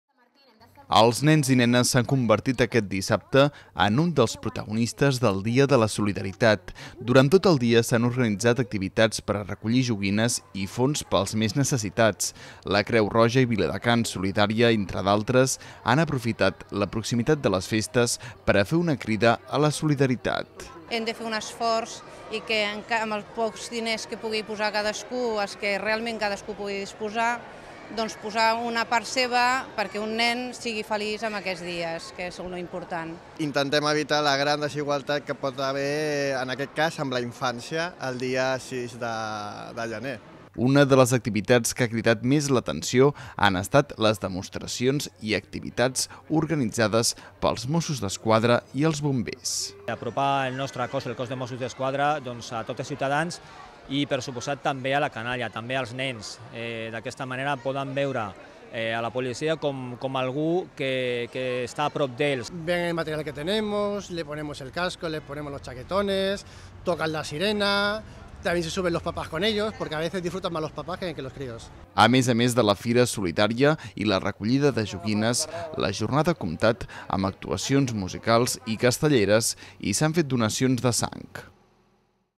El Centre Comercial Vilamarina celebra una jornada solidària
Els nens i les nenes s’han convertit aquest dissabte en un dels protagonistes del Dia de la Solidaritat del Centre Comercial Vilamarina. Durant tot el dia s’han organitzat activitats per a recollir joguines i fons pels més necessitats. La Creu Roja i Viladecans Solidària entre d’altres han aprofitat la proximitat de les festes per a fer una crida a la solidaritat.

Una de les activitats que ha cridat més l’atenció dels petits han estat les demostracions i activitats organitzades pels Mossos d’Esquadra i el cos de Bombers. A més a més de la fira solidària i la recollida de joguines, la jornada ha comptat amb una botifarrada, actuacions musicals, xocolatada, castellers i s’han fet donacions de sang en una unitat mòbil.